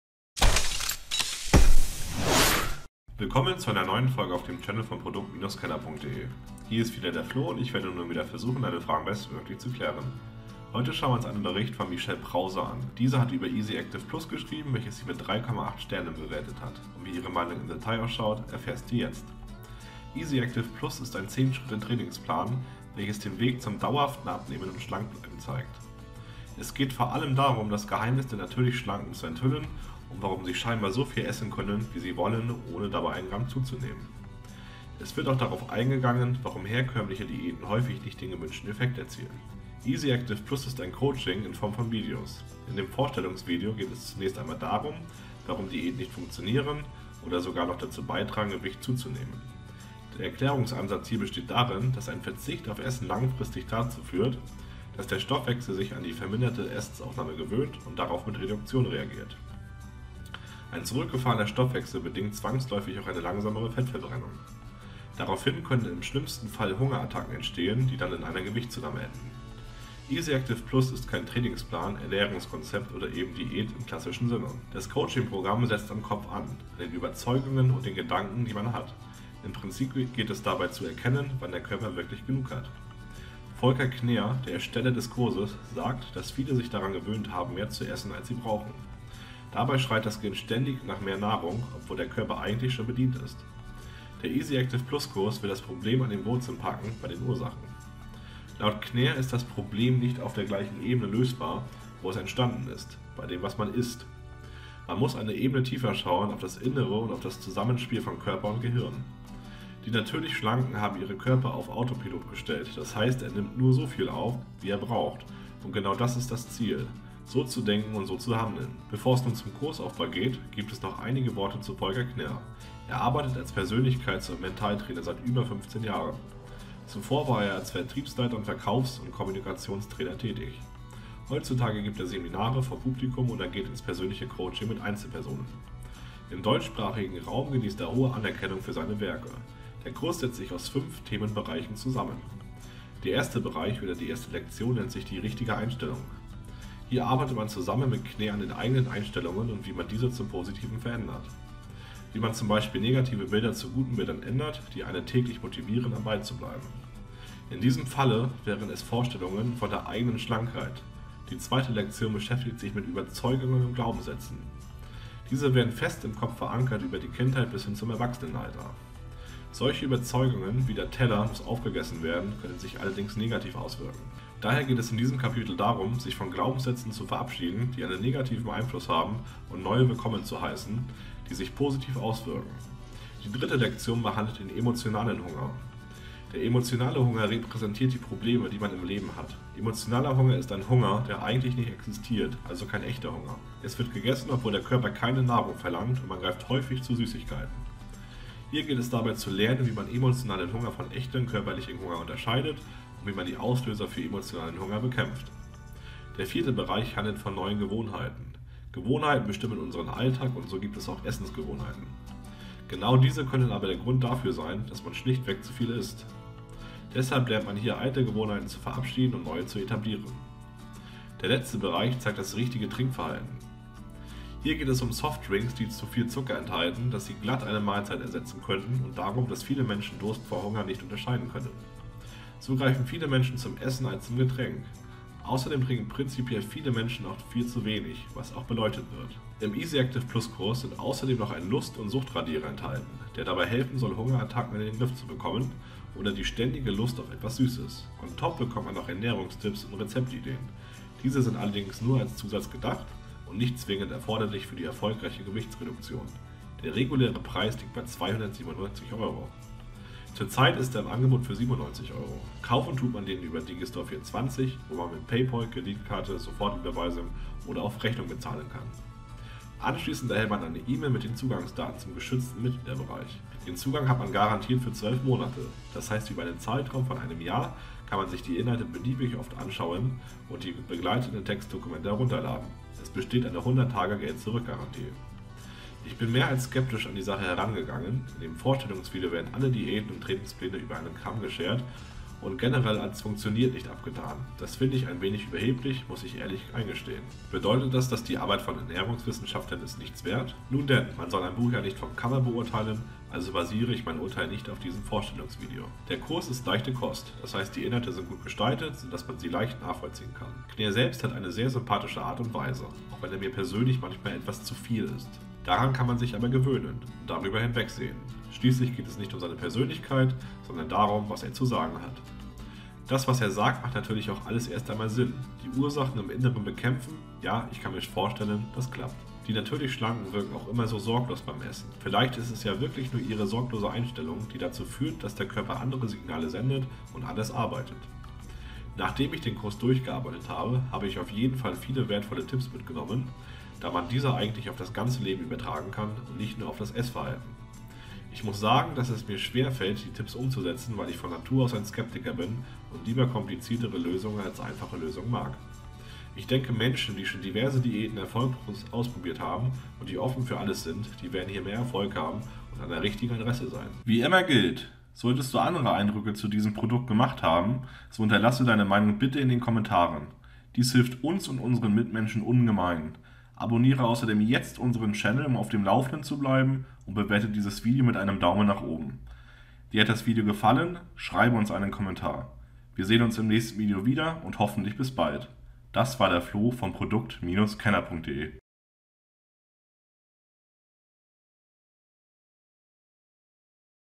[FAZIT] Easy Active Plus Erfahrungen + Review | Abnehmen mit Volker Knehr im Test!
EasyActivePlus Erfahrungen. EasyActivePlus ist ein Coaching Programm in Form eines 10-Schritte Trainingsprogramms. Alle Details dazu erfahrt ihr im Video!

EasyActivePlus ist keine Diät, kein Trainingsplan oder etwas in die Richtung. Viel mehr ist es ein Training für den Kopf. Volker Knehr, der Macher des Coachings, ist der Überzeugung, dass man eine
Ebene tiefer das Problem angehen muss, um es an den Wurzeln endgültig zu zerstören. Laut dem Autoren haben sich viele Menschen daran gewöhnt mehr zu essen, als der Körper wirklich
braucht. Um abzunehmen muss man daher an seinen Einstellungen und Überzeugungen arbeiten. Das Coaching soll einen wieder auf den richtigen Kurs bringen, nämlich zu erkennen, wann der Körper genug hat.

Dafür teilt sich EasyActivePlus in fünf Module auf. Am Anfang steht die eigene Einstellung an der gearbeitet wird. Wie kann man negative Bilder in positive verwandeln? Dann kommen Glaubenssätze und Überzeugungen an die Reihe. Zunächst werden negative Überzeugungen ausradiert, um Platz für neue zu machen mit positiven Auswirkungen. In der dritten Lektion wird der „emotionale Hunger“ thematisiert, der Probleme darstellt. Emotionaler Hunger führt dazu, dass man isst ohne wirklichen echten „körperlichen“ Hunger zu haben. Dafür müssen häufig Süßigkeiten herhalten. Man lernt hier emotionalen, unechten Hunger von echten Hunger zu unterscheiden. Das vorletzte Modul geht auf Gewohnheiten ein und wie man neue Gewohnheiten in den Alltag integrieren kann. Eine besondere Rolle spielen hierbei natürlich Essensgewohnheiten. Im letzten Modul wird von dem richtigen Trinkverhalten gesprochen. Leider greifen viele Menschen zu Softdrinks anstelle zu Lebensmitteln, weil Durst und Hunger missinterpretiert wird.

Im EasyActivePlus bekommt man außerdem noch zahlreiche Rezeptideen und allgemeine Ernährungstipps.

Wenn Du mehr zu Easy Active Plus erfahren willst, dann schau JETZT bei Produkt-Kenner.de vorbei!